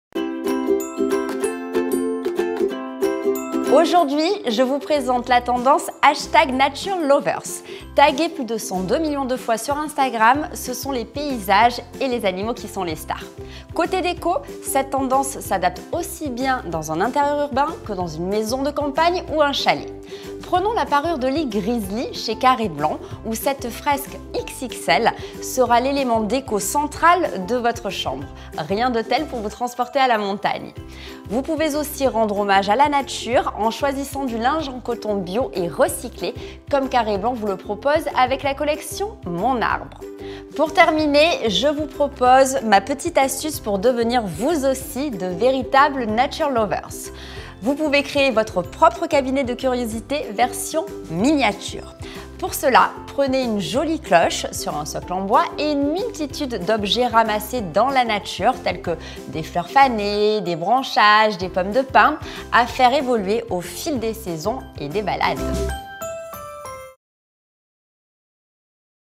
NatureLovers : la tendance déco décryptée par Emmanuelle Rivassoux
Ambassadrice de marque pour Carré Blanc, Emmanuelle Rivassoux nous explique comment adopter la tendance NatureLovers en invitant la nature dans notre décoration intérieure.